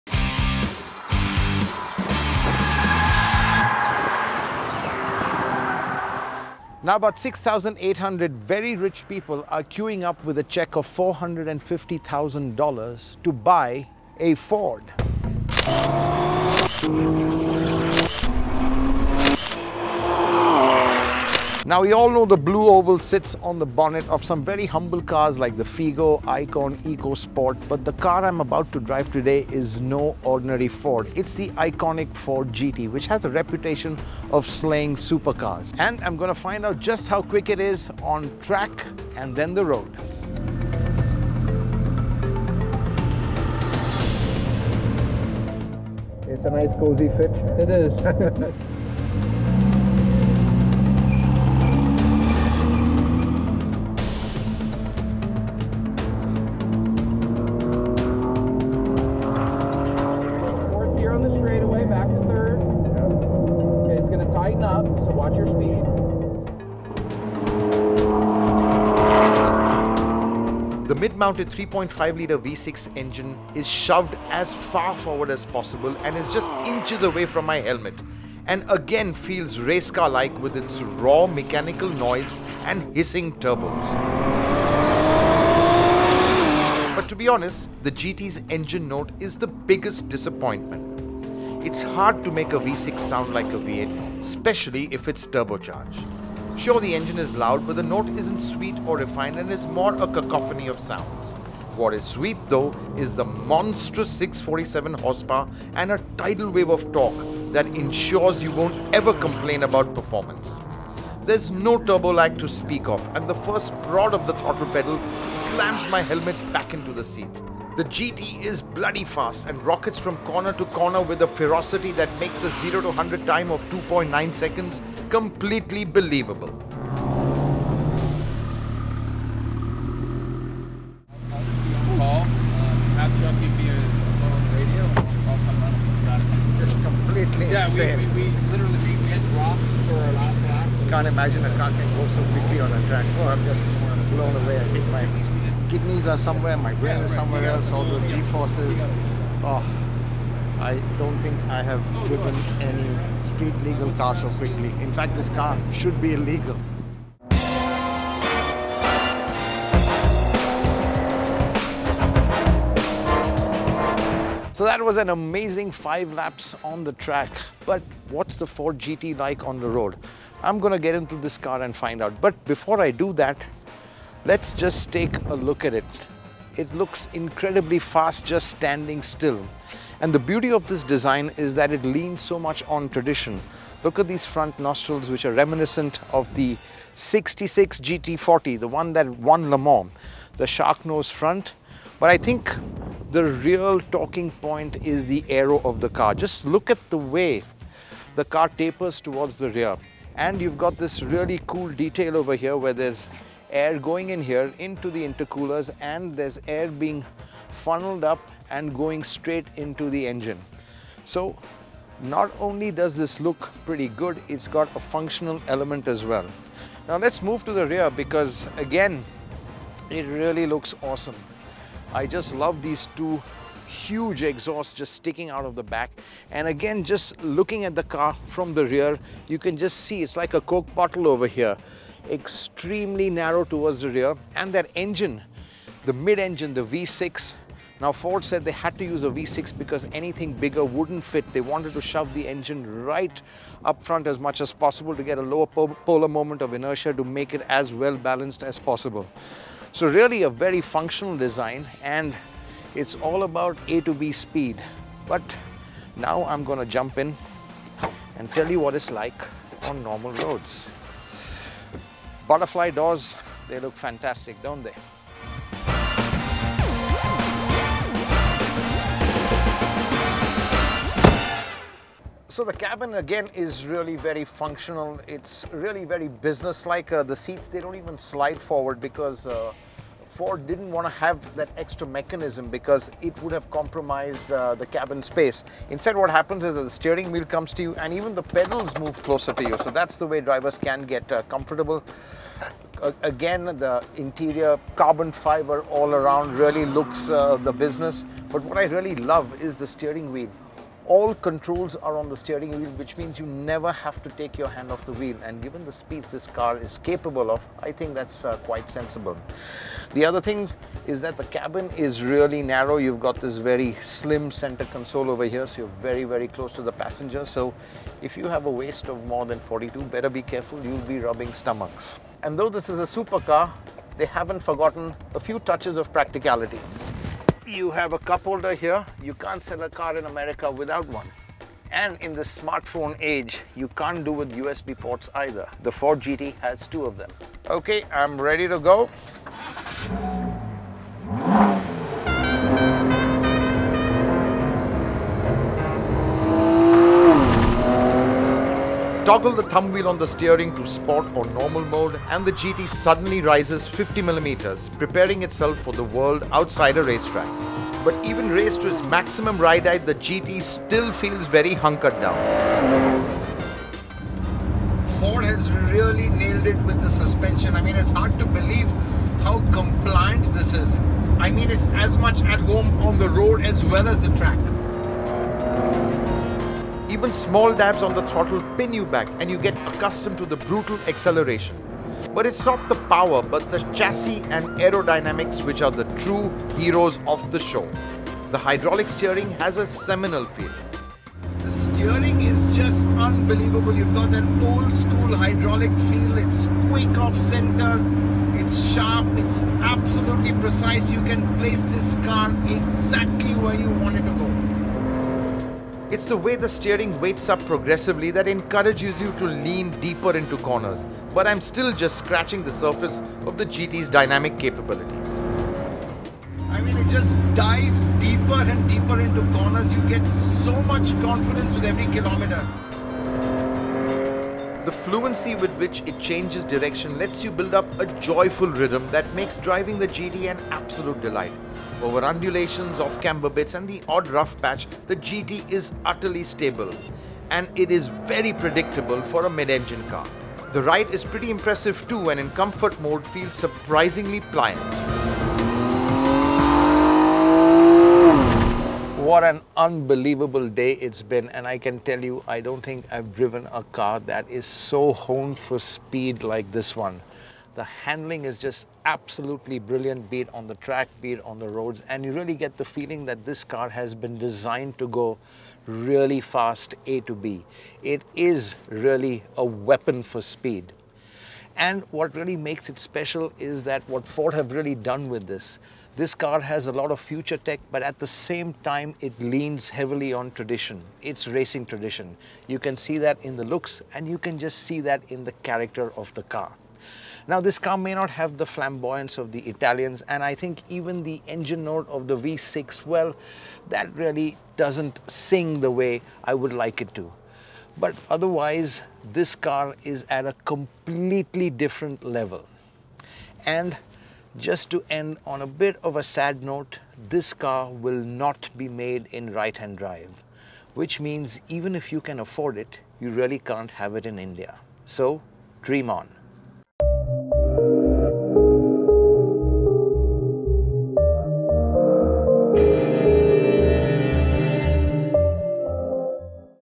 FORD GT | First Drive | Autocar India
Hormazd Sorabjee is the only Indian Journalist to drive the new Ford GT, a race car that's built for the road.